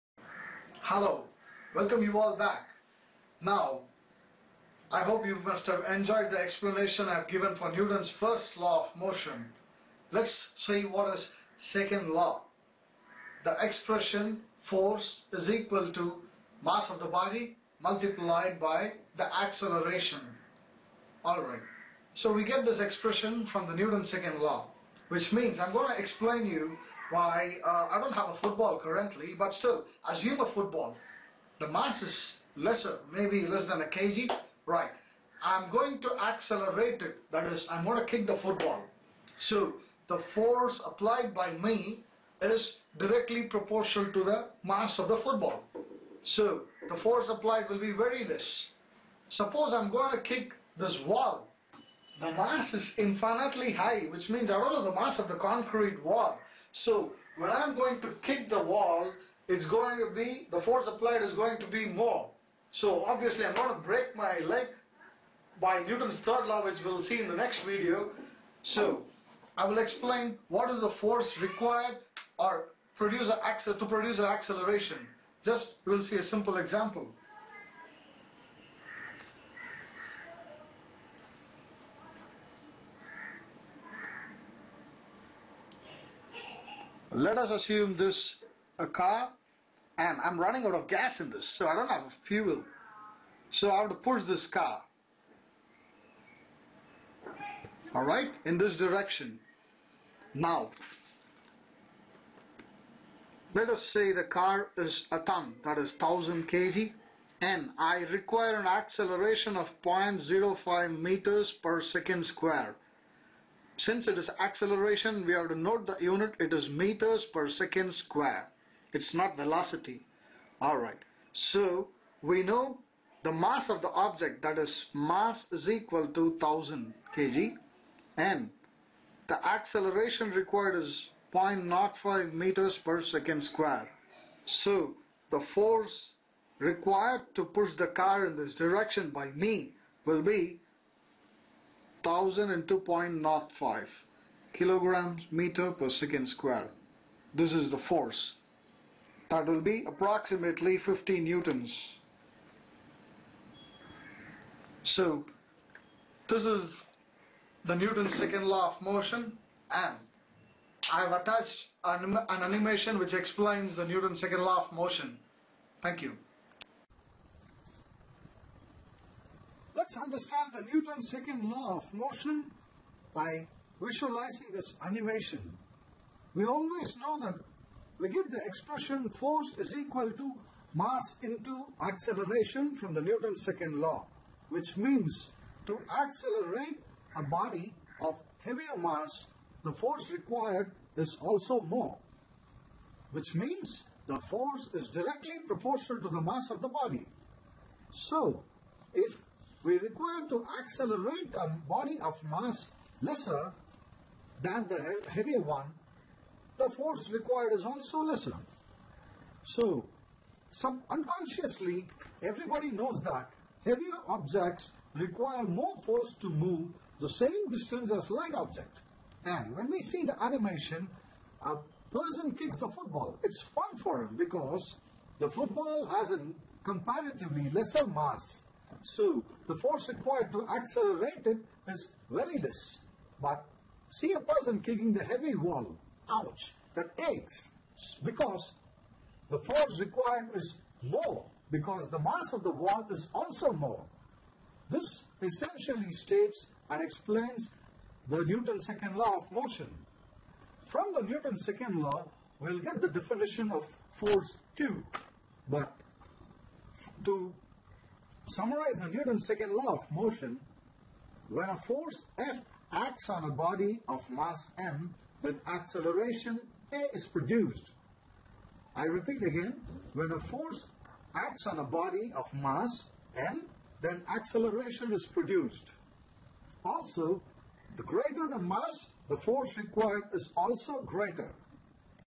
Newtons 2nd law - Explained with Examples & Animation.
Hello Kids, This video will explain about the Newton's 2nd Law of Motion with examples and animation.